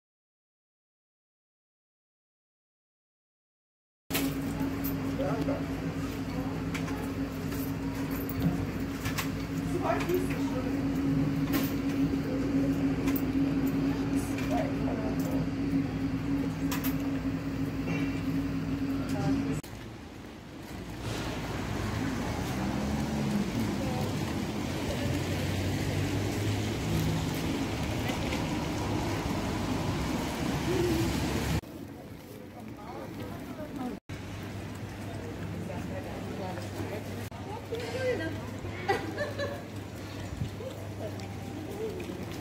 Frankfurt: Leipziger Straße während Corona-Krise
Die Leipziger Straße in Frankfurt Bockenheim, als in der Corona-Krise die Geschäfte wieder öffnen durften. Lange Schlangen vor Zalando!